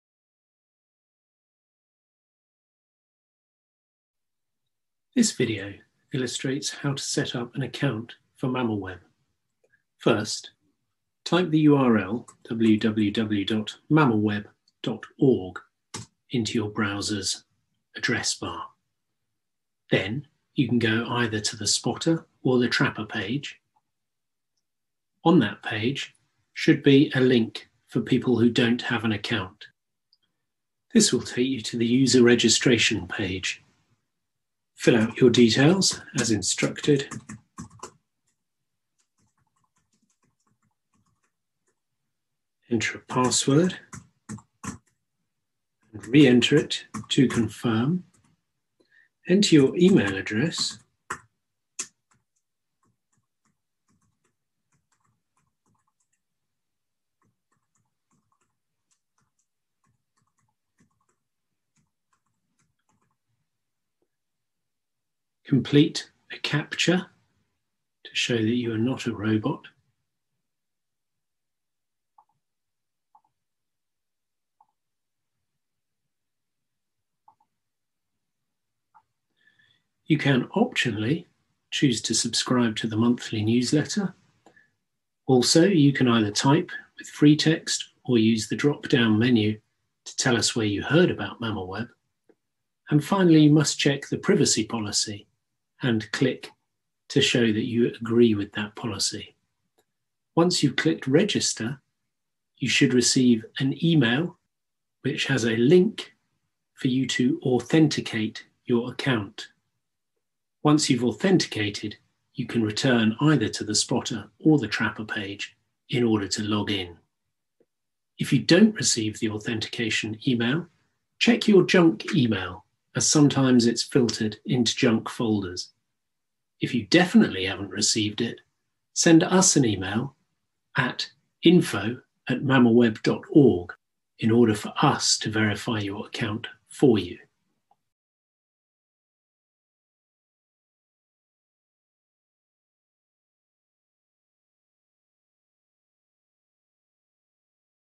How to get an account for MammalWeb
This video is about registering for MammalWeb